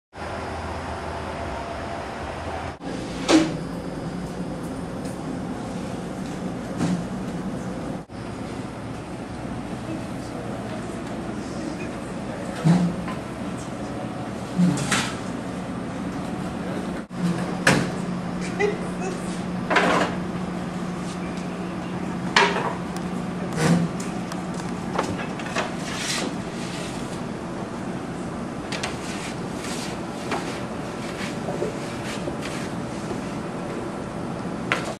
Procolino's
Short walk through of Procolino's pizzeria